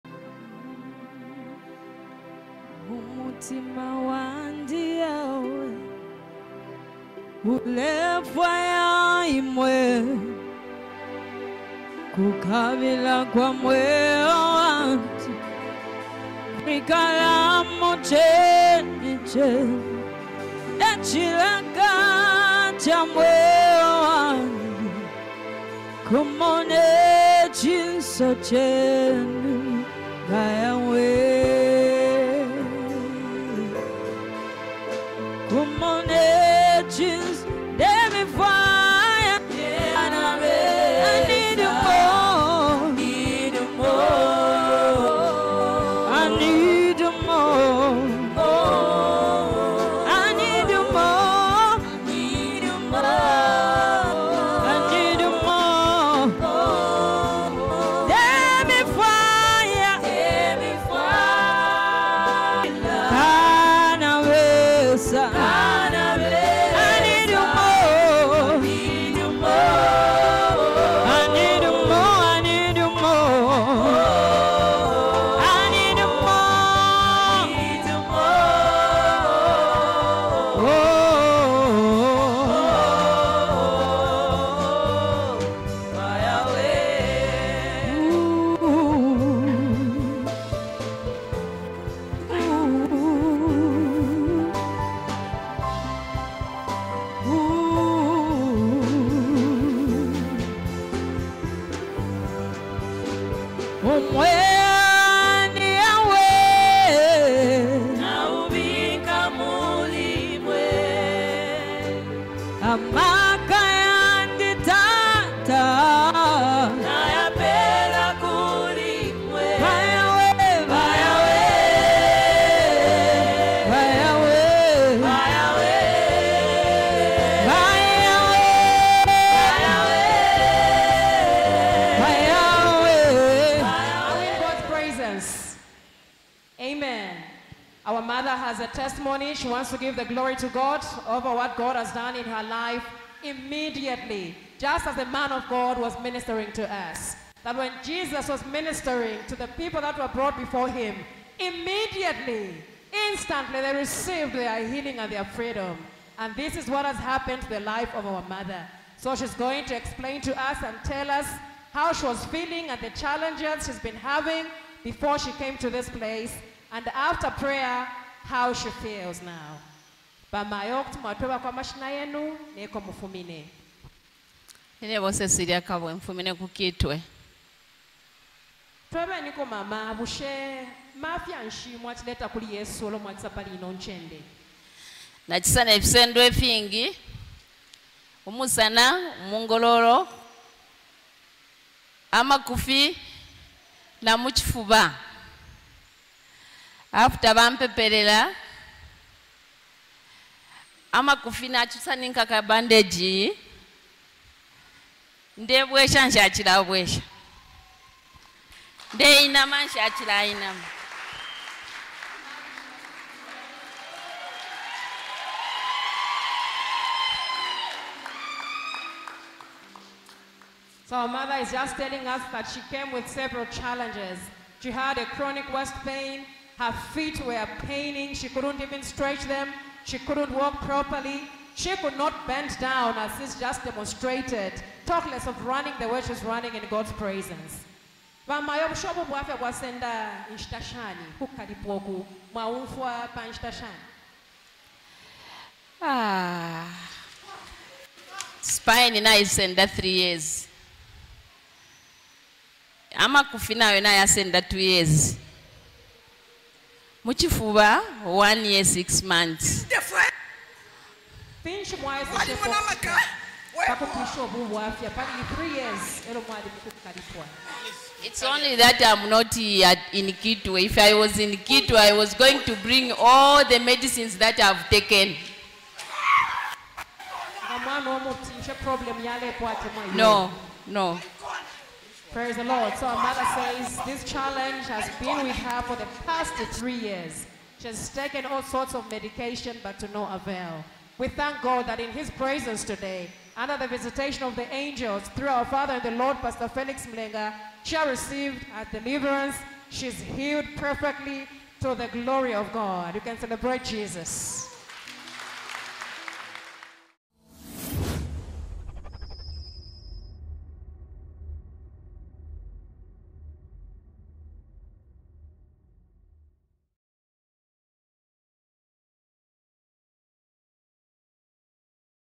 THIS IS WHAT HAPPENS WHEN YOU JUST BELIEVE IN GOD
Where the spirit of God is, there is liberty. CECILIA KABWE has received her freedom from pain and affliction.
 For three years Cecilia visited several hospitals in search of healing but to no avail. She suffered from severe backache, waist pain, chest pain, and knee problems.
 She could not walk properly, bend or sit freely because of the pain she was experiencing. But after the servant of God pastor Felix Mulenga prayed for her, she received her healing immediately. What a mighty God we serve!

Prayer Lines:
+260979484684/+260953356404